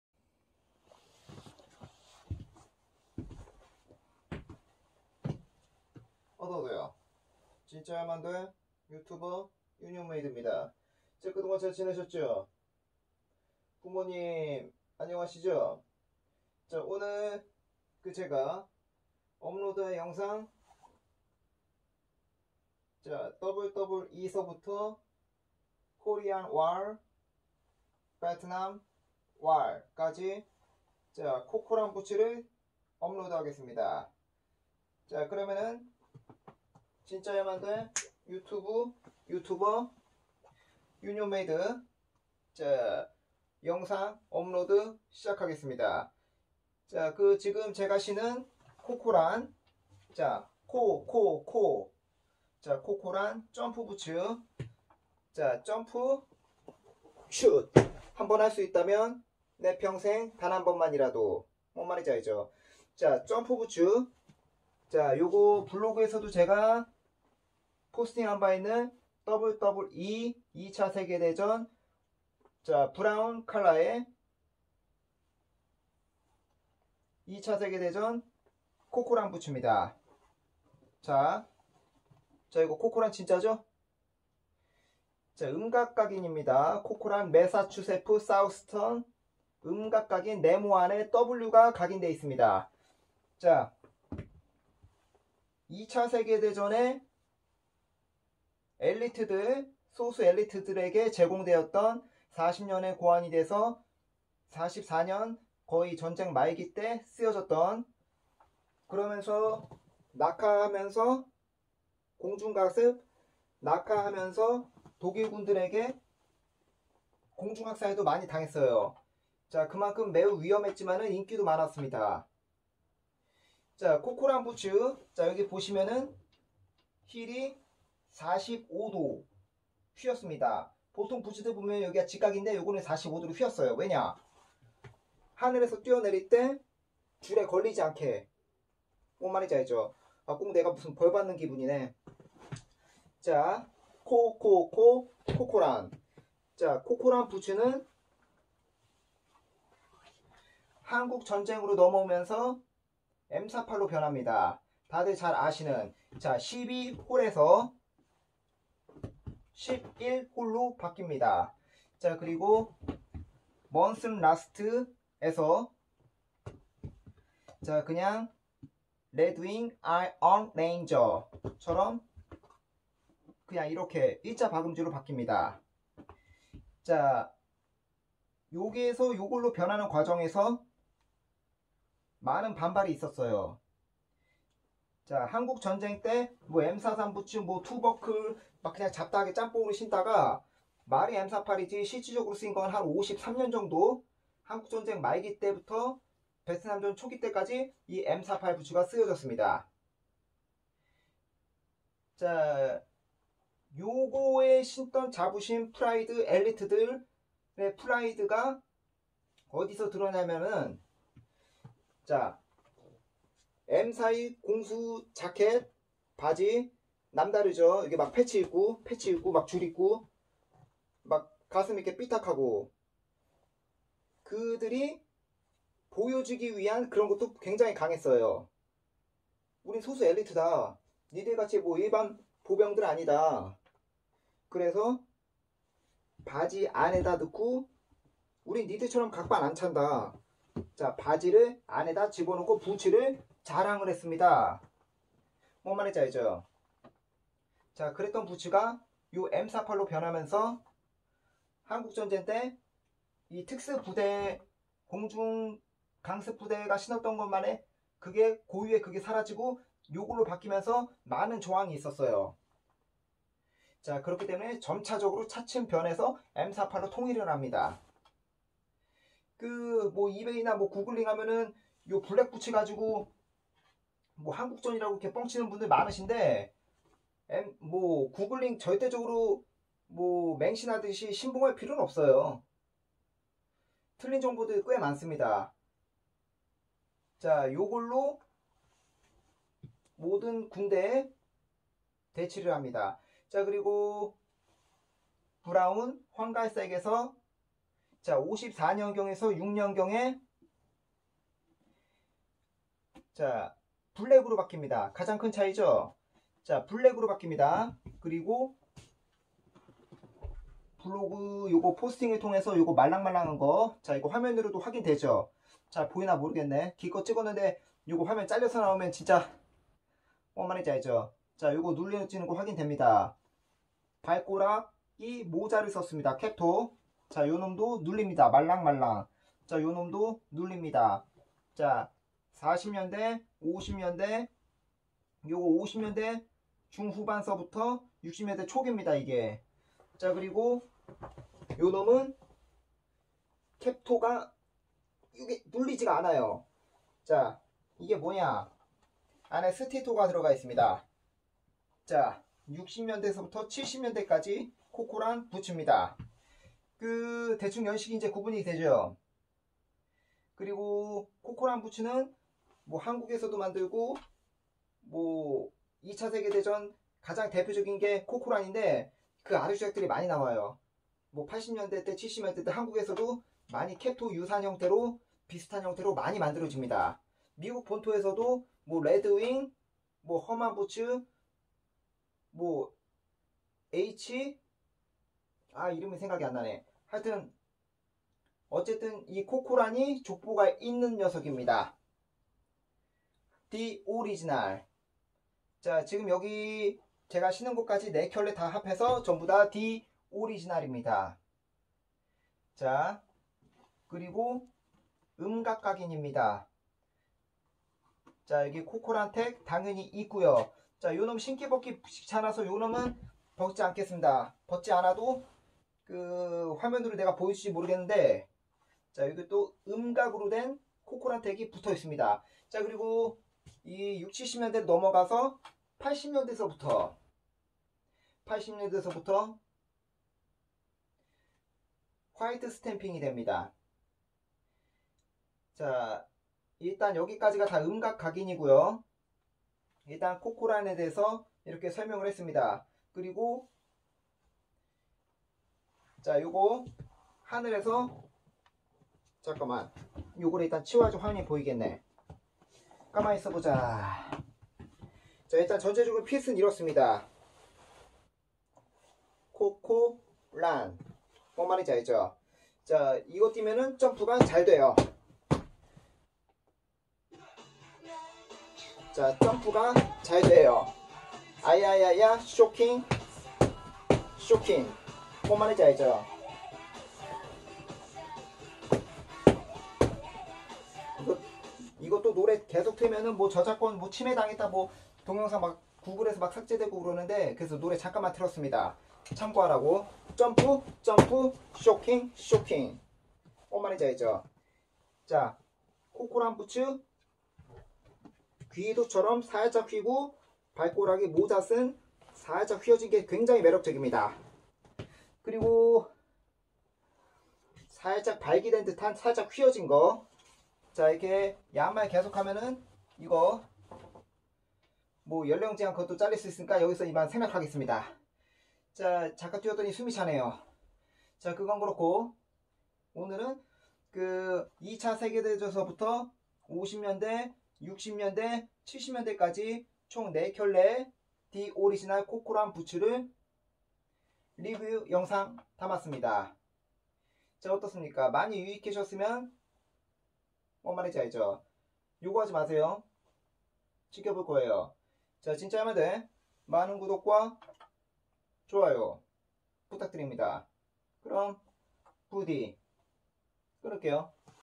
WW2~Korea.Vietnam War Corcoran Jumpboots 이차대전부터 한국전 배트남전 코코란점프부츠리뷰
전군의 선망의 대상이였던 이차대전 코코란 점프부츠와 한국전 대트남전 코코란부츠까지 리뷰했습니다